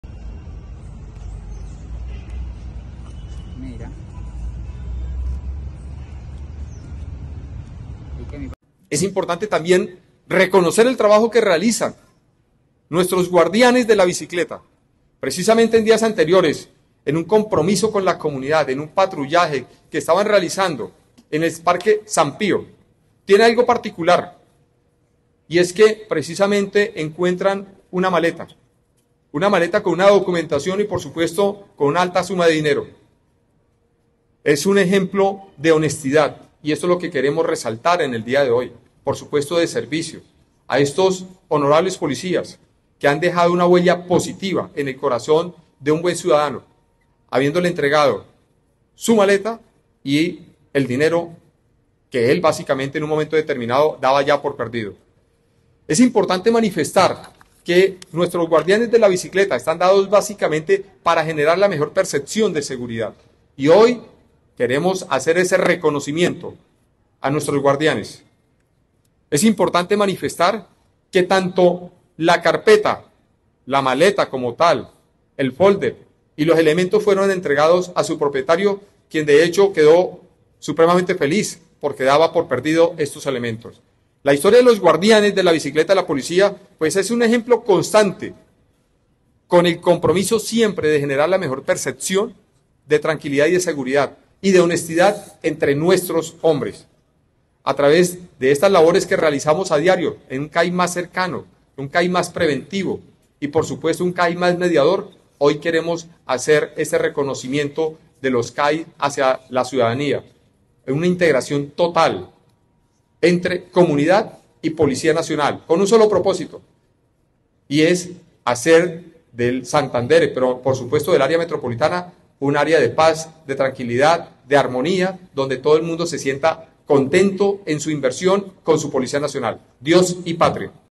Encontraron una plata tirada y se la devolvieron a su dueño en Bucaramanga
Los guardianes en bicicleta de la Policía en Bucaramanga se encontraron tirada una carpeta en la que habían importantes documentos y una cuantiosa suma de dinero. Fue devuelta a su propietario.